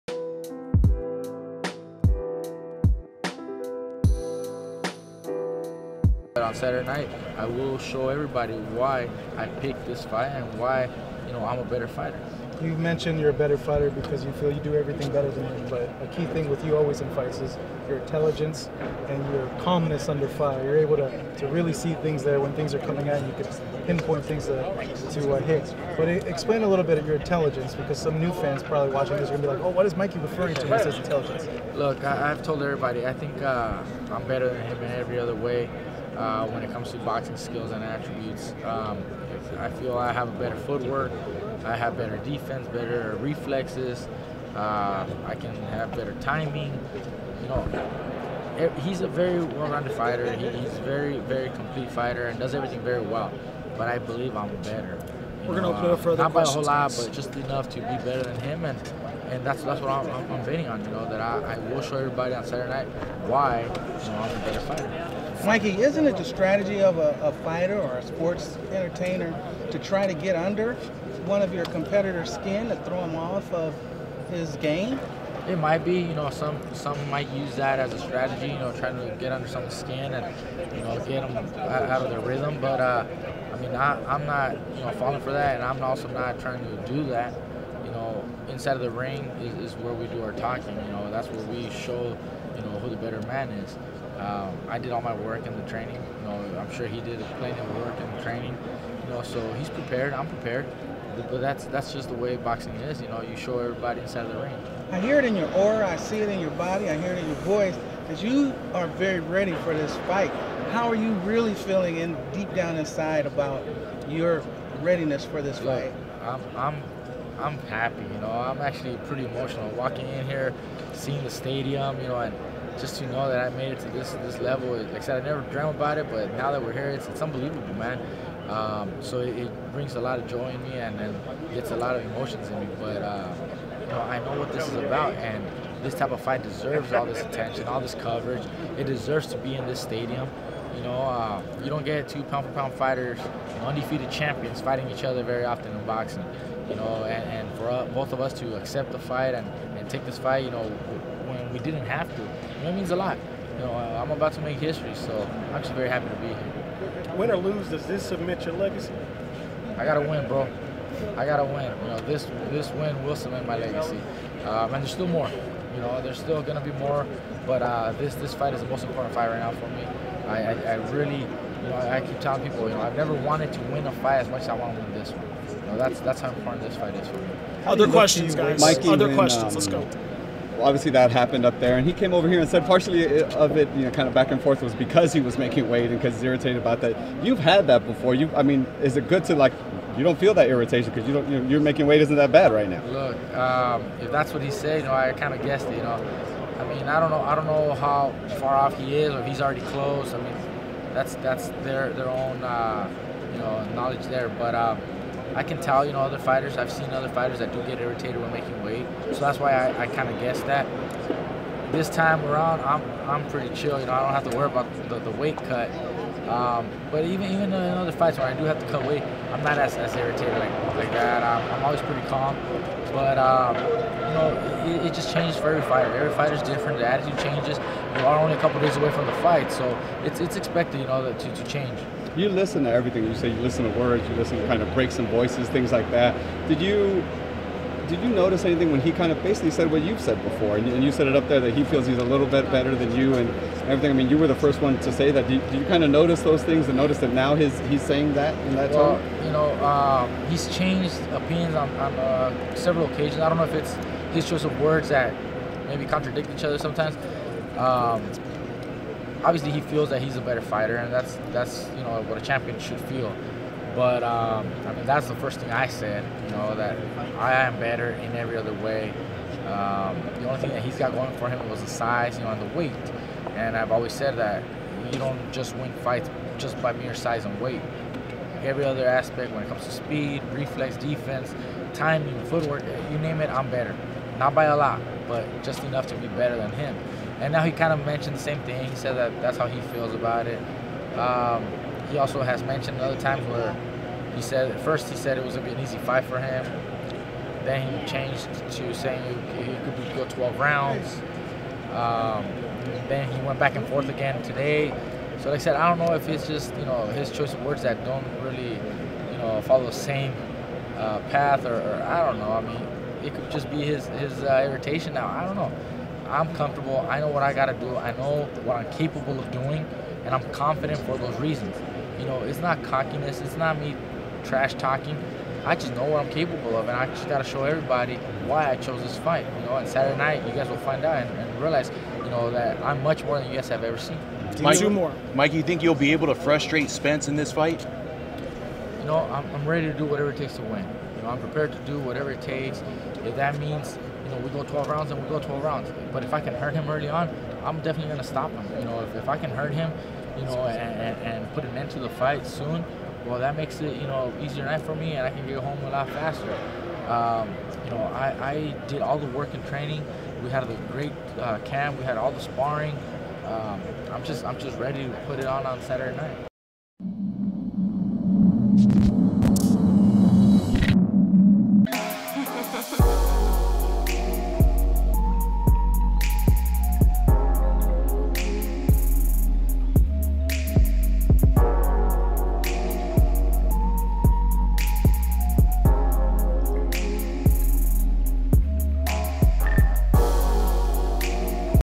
Mikey Garcia on Getting Under Errol Spence's Skin & Says Errol Spence is Contradicting His Words!
Watch as Mikey Garcia speaks on Getting Under Errol Spence's Skin & Says Errol Spence is Contradicting His Words!